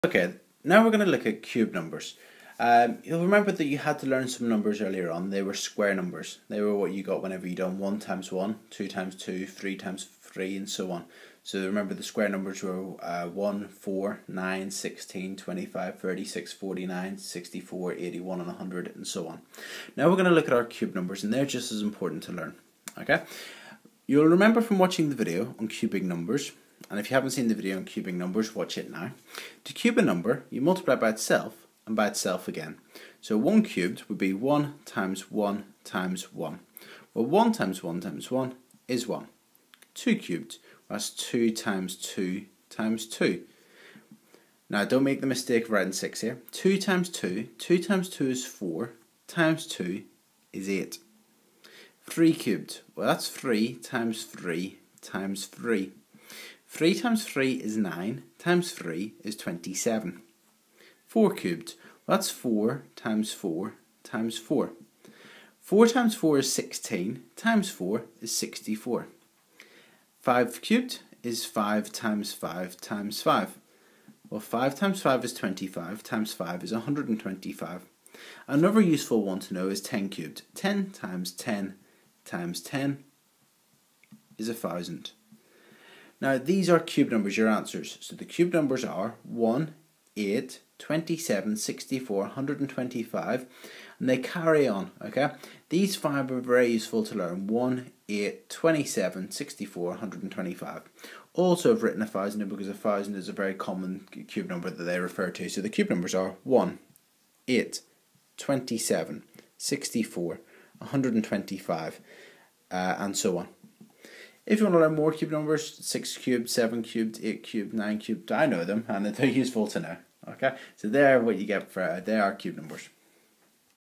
Cube Numbers - Corbettmaths
This video explains what cube numbers are and how they are worked out.